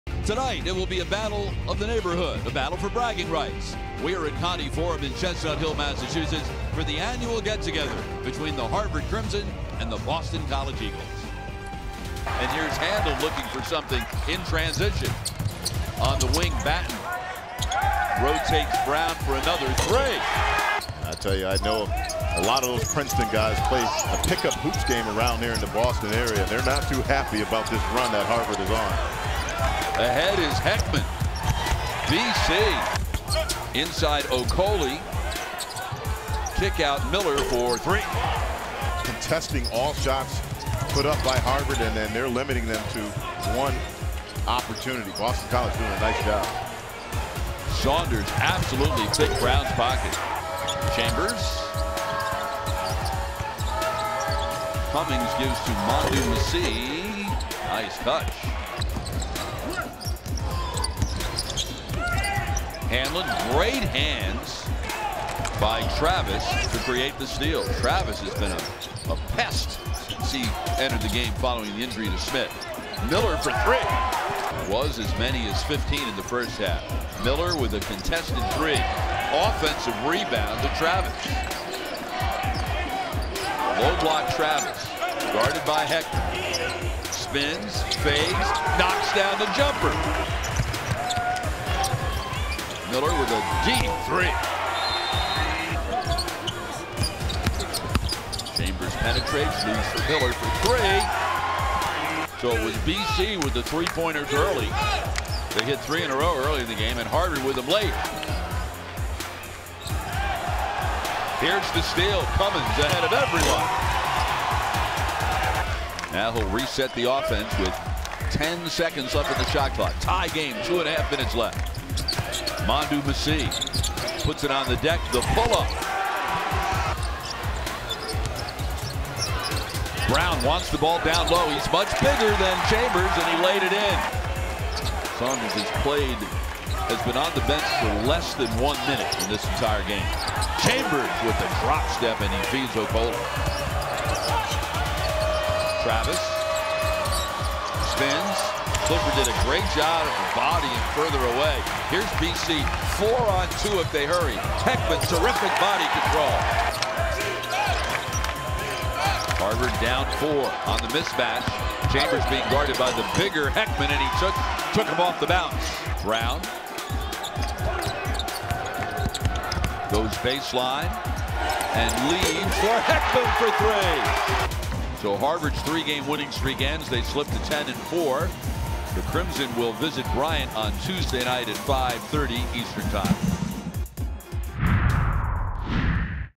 Game Recap: Men's Basketball's Comeback Bid Falls Short in Overtime at Boston College
Wesley Saunders scored 15 points and Corbin Miller added 14 as the Crimson fell in overtime at Boston College, 64-57, despite overcoming a 15-point deficit.